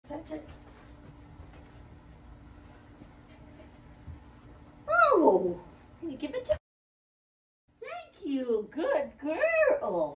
DMWYD Dog Olympics: Fetch My Handkerchief
2024 DMWYD limited series offering a new specialty title challenge for 2024. The fourth challenge is "Fetch." Jeep needs to demonstrate ten tricks fetching something or someone. In this trick Jeep Fetches a Handkerchief from my back pocket.

Join the Tricks with Jeep & Ticket Spark Team on Facebook to have your tricks evaluated free to earn titles in DMWYD.